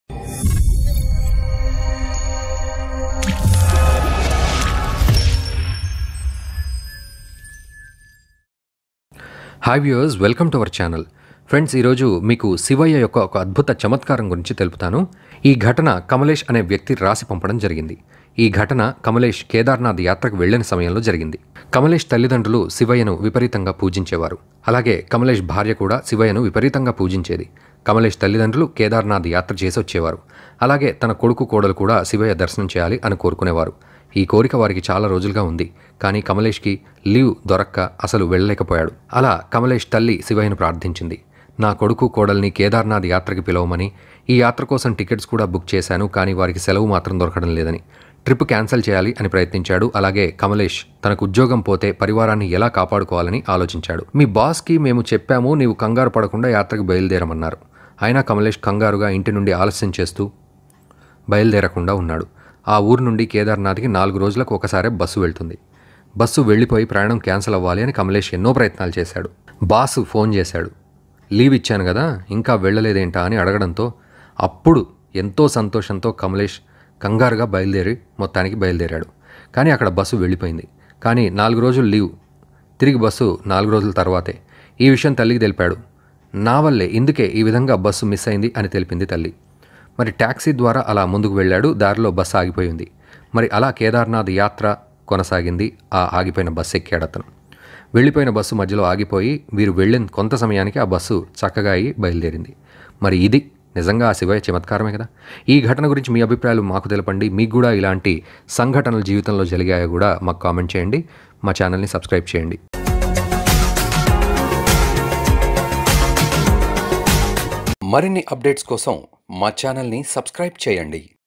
కేదర్నాథ్ కి వెళ్తున్న బస్సుని రోడ్డు మధ్యలో ఆపిన శివుడు..ఎందుకో కారణం తెలిస్తే బిత్తరపోతారు Shiva
Watch: కేదర్నాథ్ కి వెళ్తున్న బస్సుని రోడ్డు మధ్యలో ఆపిన శివుడు..ఎందుకో కారణం తెలిస్తే బిత్తరపోతారు Shiva.